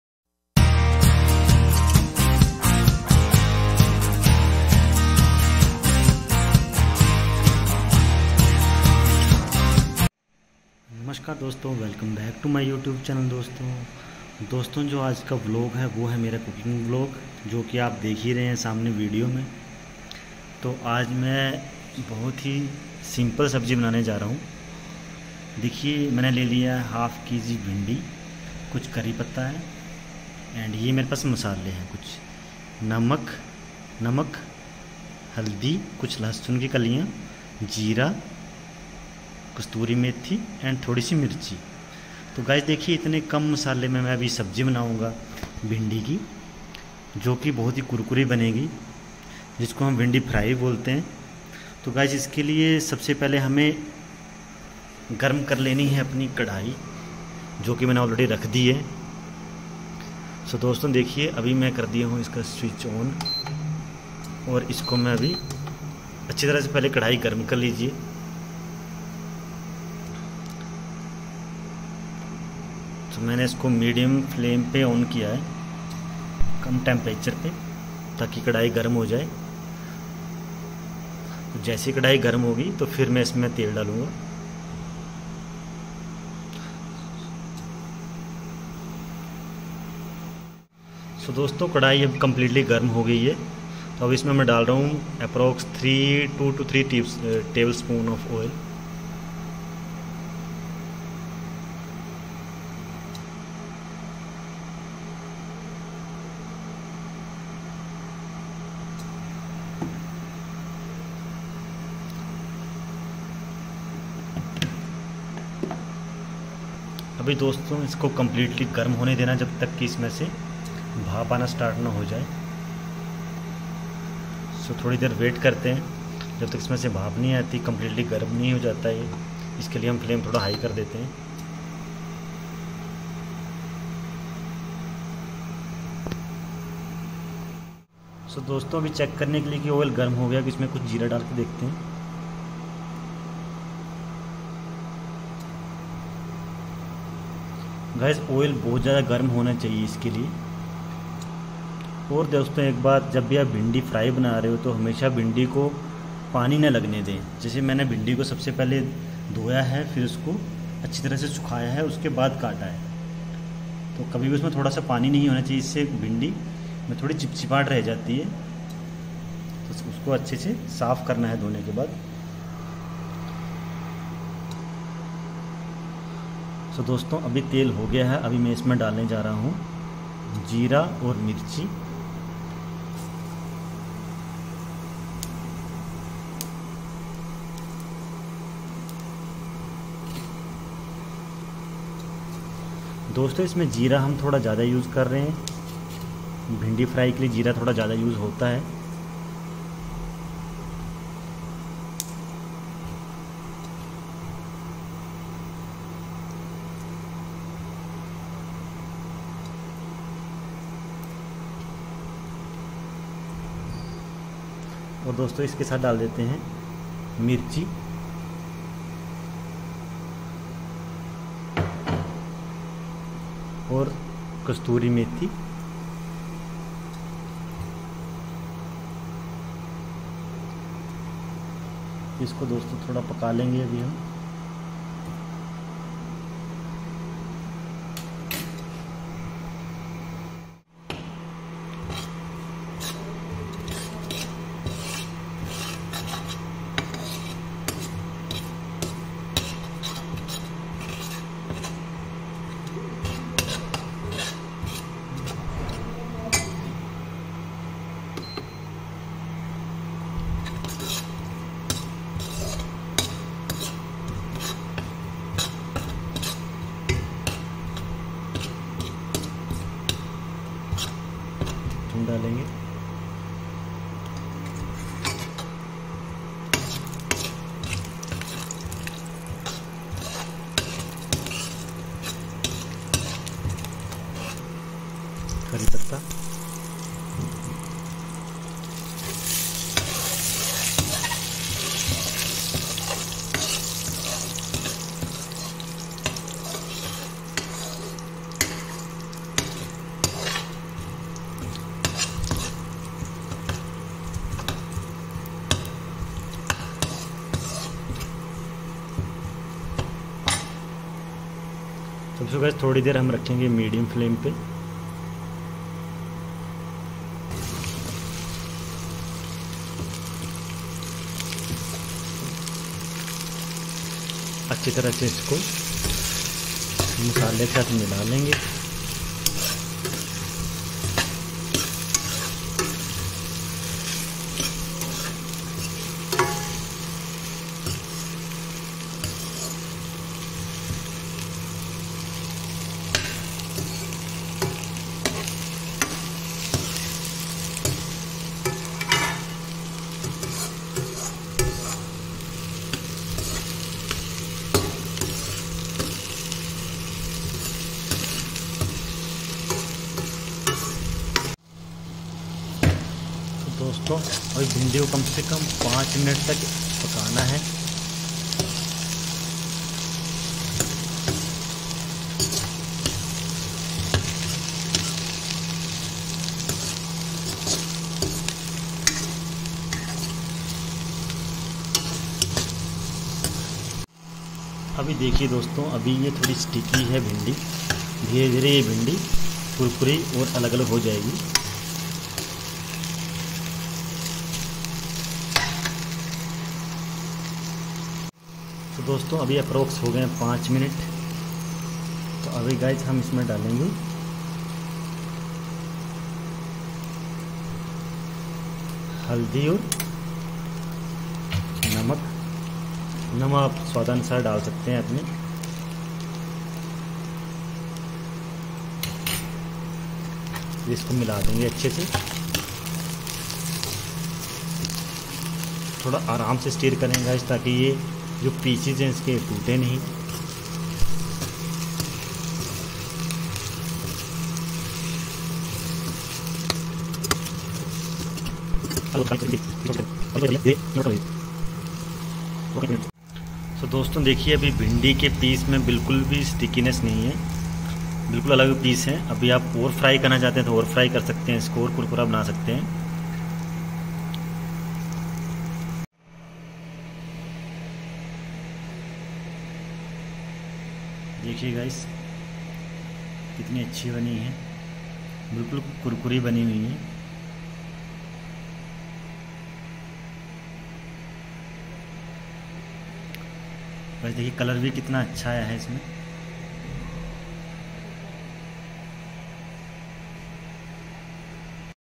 Vlog-164 इतना tasty भिंडी फ्राई मैंने आज तक नहीं खाई और वो भी इतने कम मसलों में //Cooking Vlog
पहली बार सब्जी बनाई और वो भी इतने कम मसालों में/मेरी fav भिंडी फ्राई //amazing experience